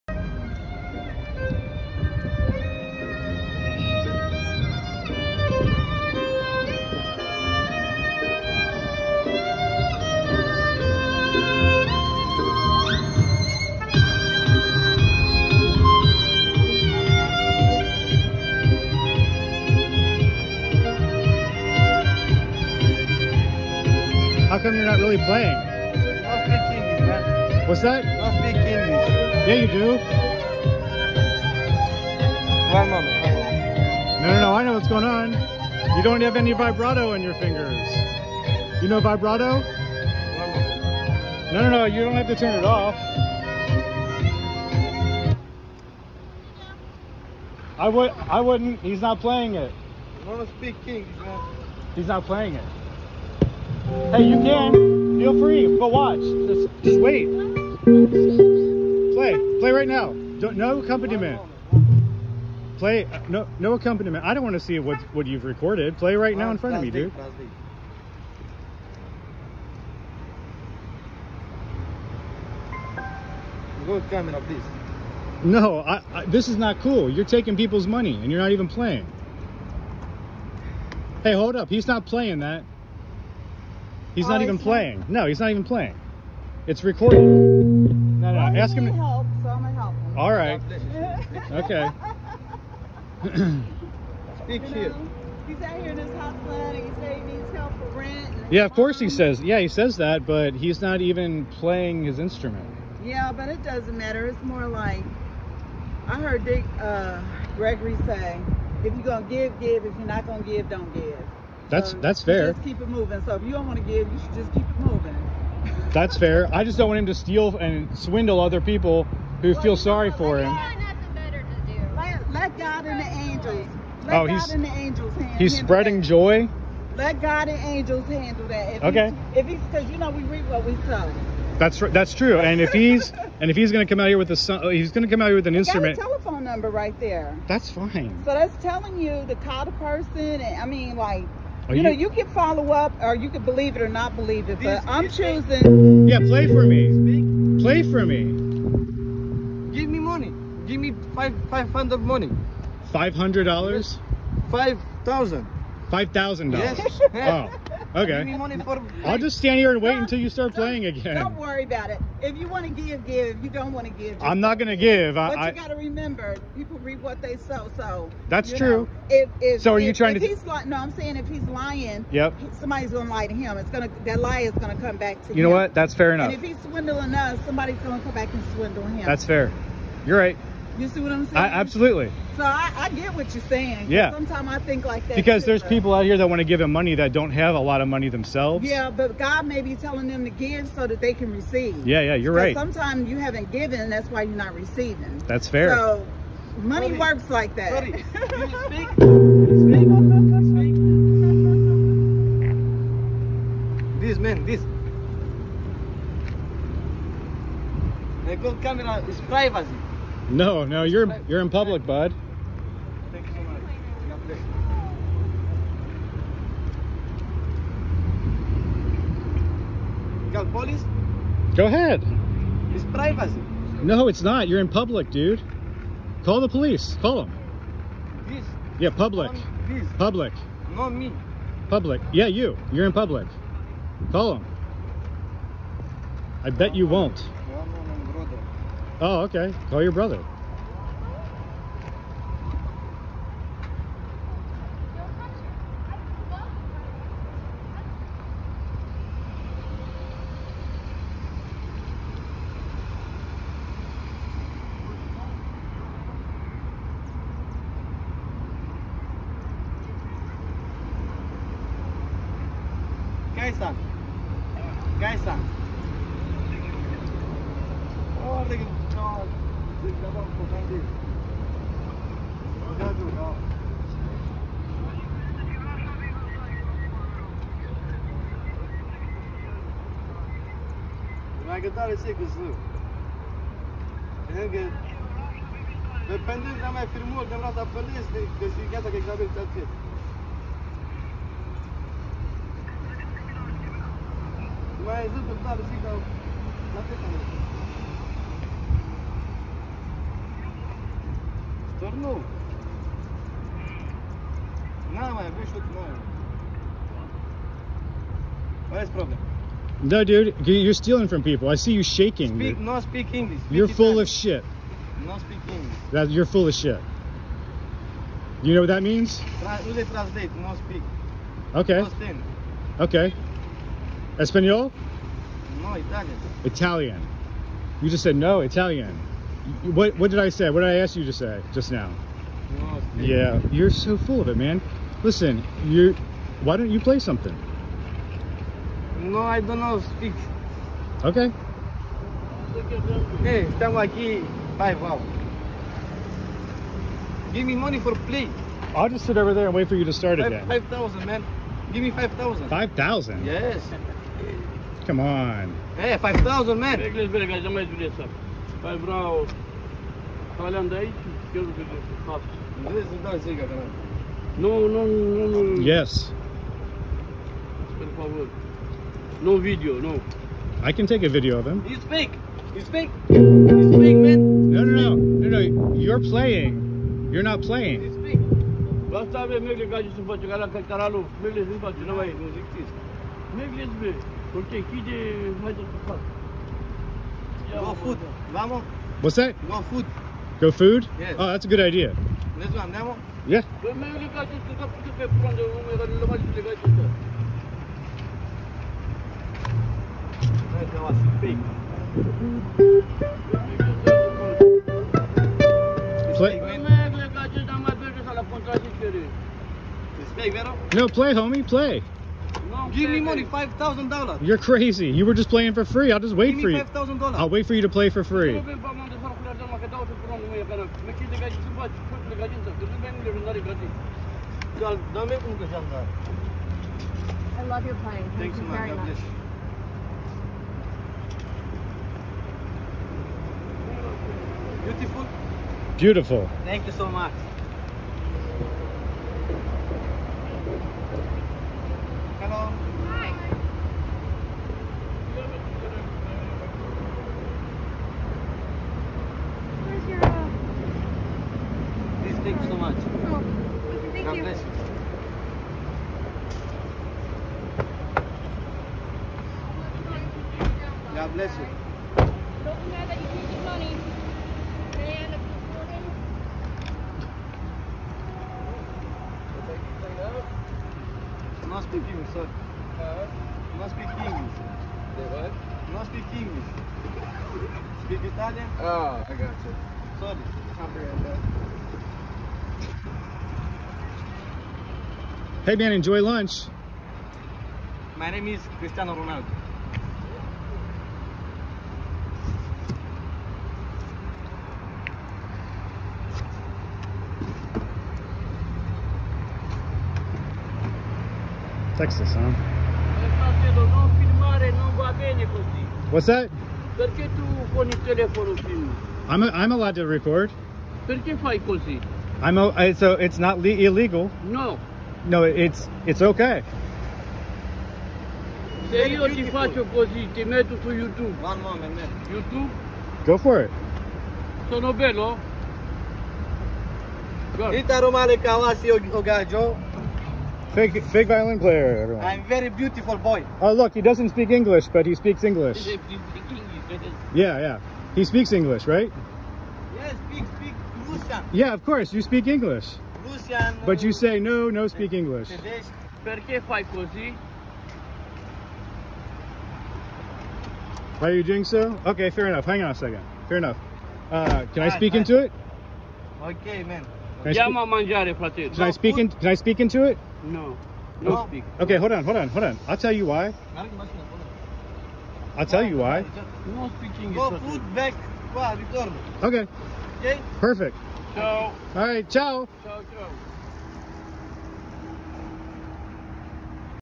Guy pretending to play violin scams people
This guy pretends to play the violin while scamming people in the community. When confronted, he decides to bail out instead of proving he can actually play like a musician with years of experience. He’ll tell you he doesn’t speak English when it’s convenient and people fall for it time and time again.

He has new clothes, shoes, hat, 2 CELL PHONES, one in each back pocket and has no concept of vibrato.